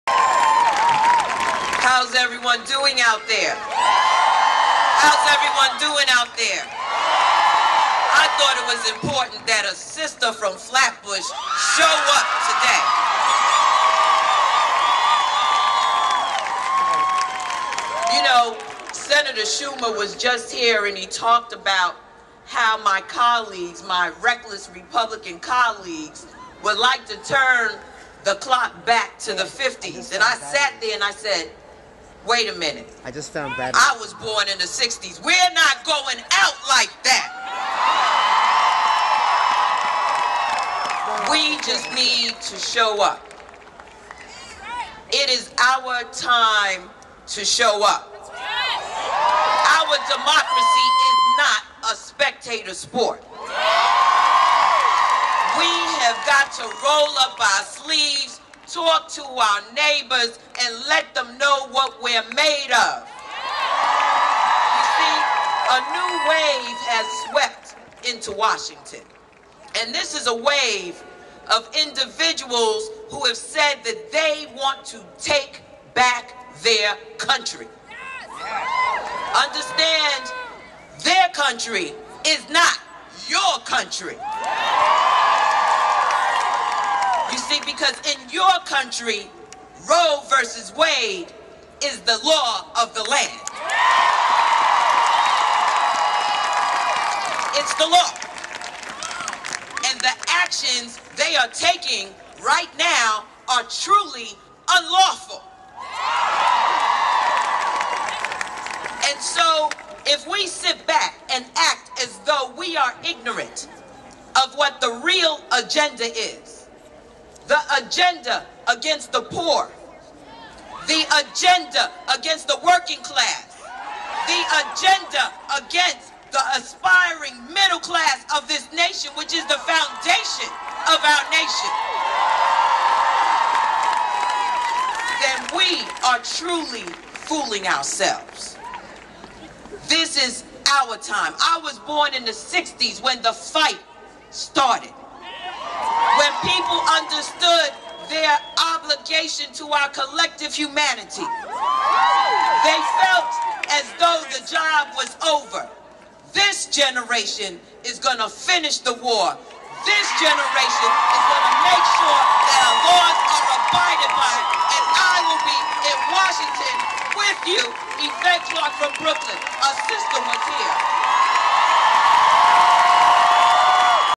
Congressmember Yvette Clarke
Congressmember Yvette Clarke speaks at Planned Parenthood Stand Up For Women's Health Rally 2011